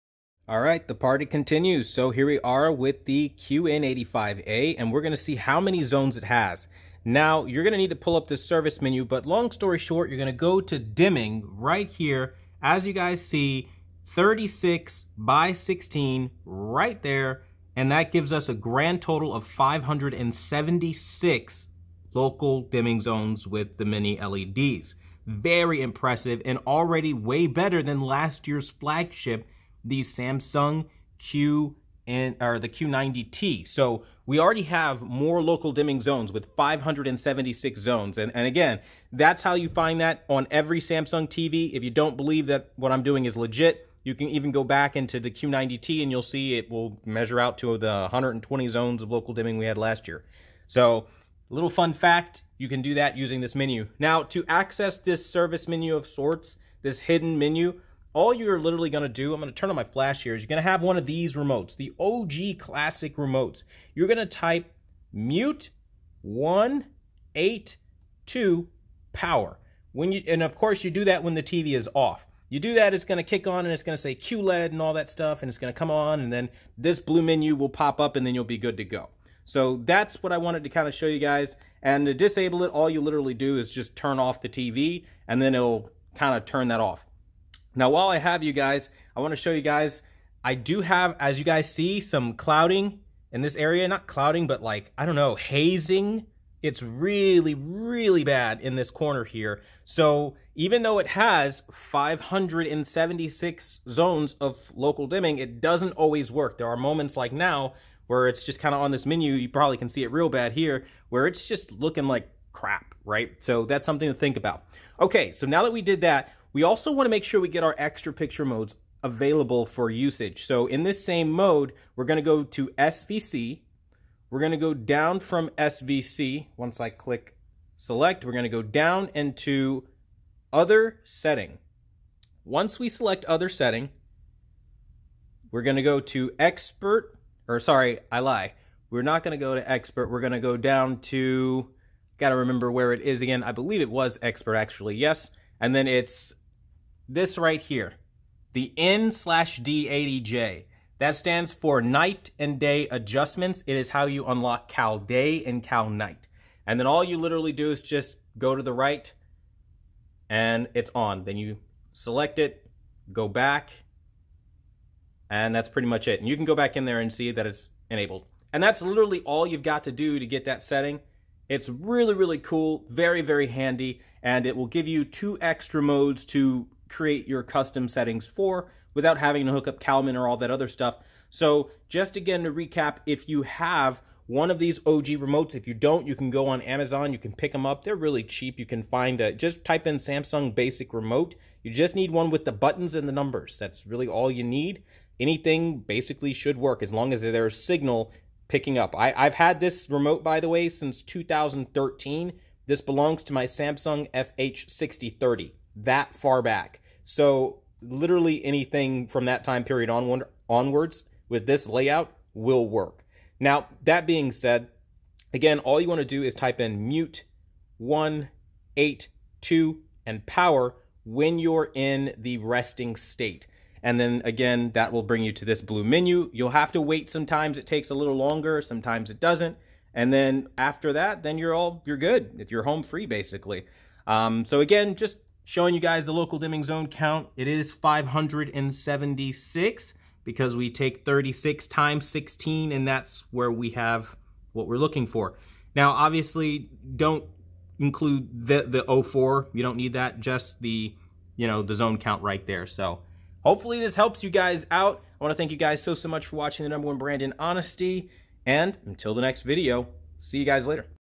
Shocking Samsung QN85A 576 Dimming Zones+CAL DAY/CAL NIGHT Activation!
▶️==General Overview==
Samsung QN85A with CAL DAY, CAL NIGHT, and where to find the official dimming zone count! I can't believe it has 576 zones! not bad at all!

Facebook.com/Quantumtvblog

Twitter.com/Quantumtvblog

▶️ SOURCES
Quantum TV

▶️ SUMMARY
Samsung QN85A Review